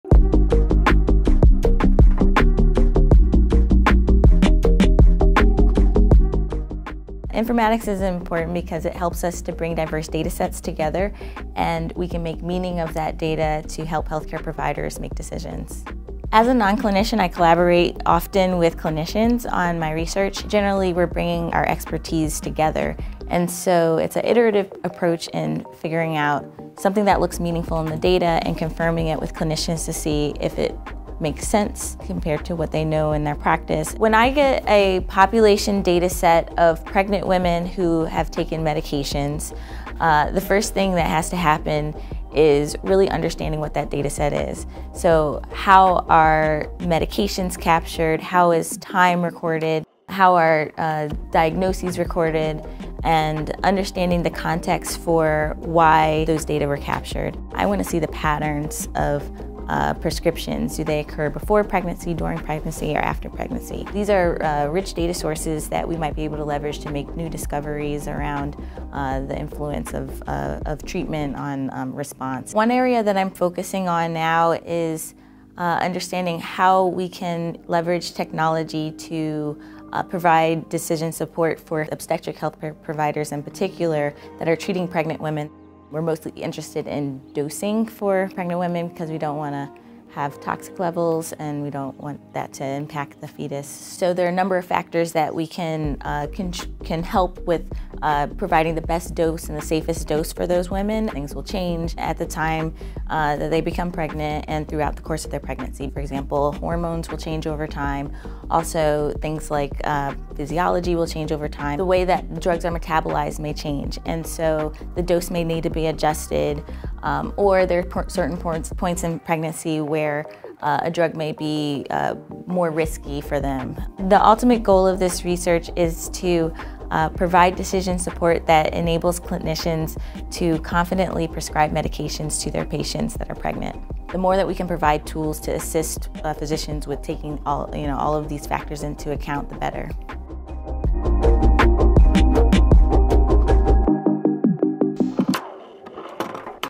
Casey Overby Taylor Informatics in Action
WhyInformatics? So, we can help clinicians confidently prescribe medications to pregnant women. -Casey Overby Taylor.